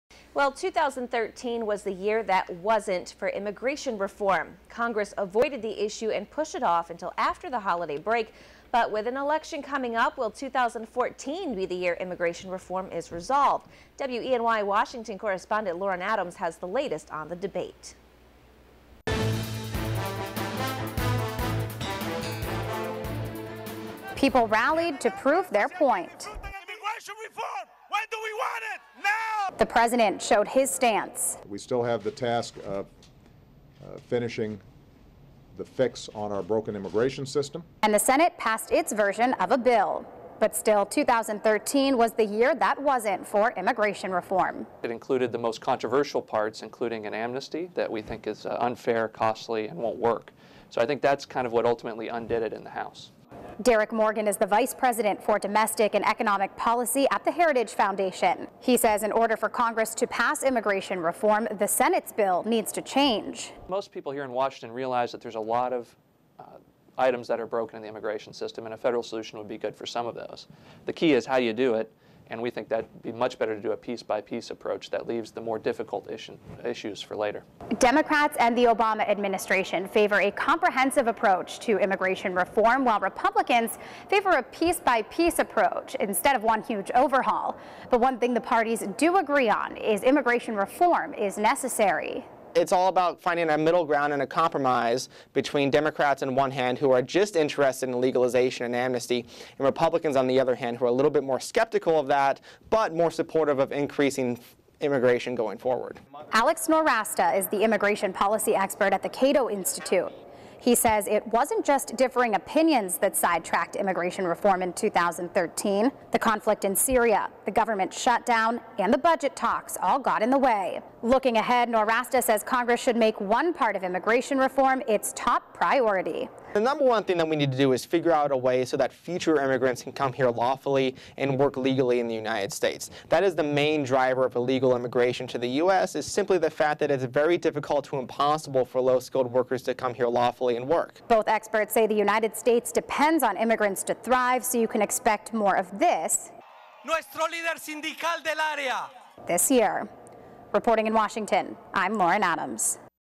Washington Bureau: Immigration Reform
Congress avoided  immigration reform last year, but with an election coming up, will 2014 be the year immigration reform is resolved?  WENY Washington Correspondent Lauren Adams has the latest on the debate.